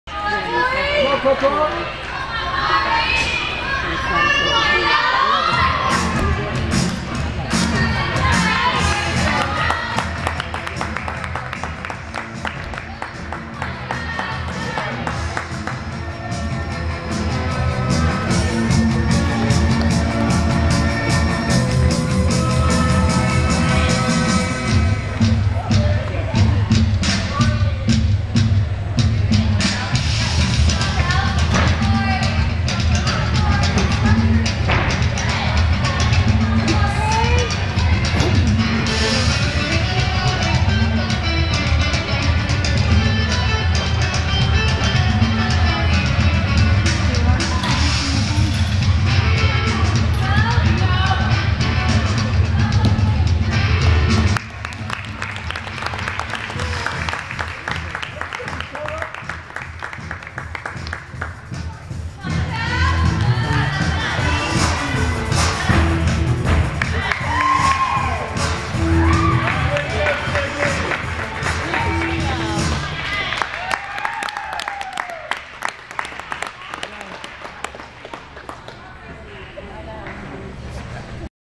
Chelsea Jones - Gymnaworld - Indiana Gymnastics Festival Level 8 All Ages Floor Exercise 01-17-2016
Chelsea Jones on Level 8 All Ages Floor Exercise in the Indiana Gymnastics Festival at Triton Central High School on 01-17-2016
(5th Place)
(5th Place -  All-Around)